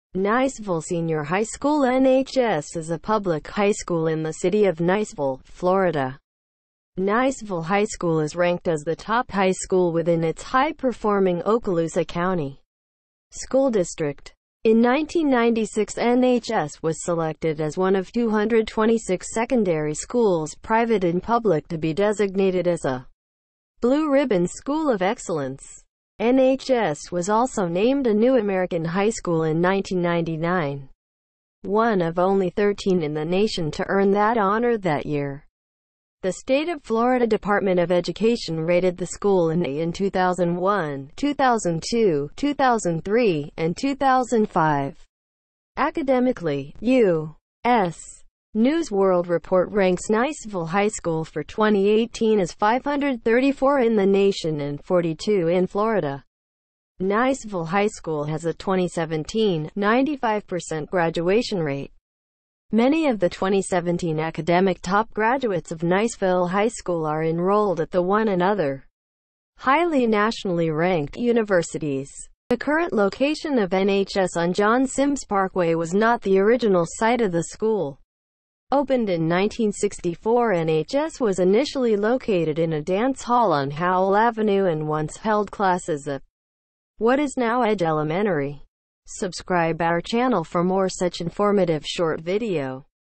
Learn about niceville high school | what is afjrotc
A detail information about what is afjrotc. I do not own it. I just make video out of this so that some people get some help.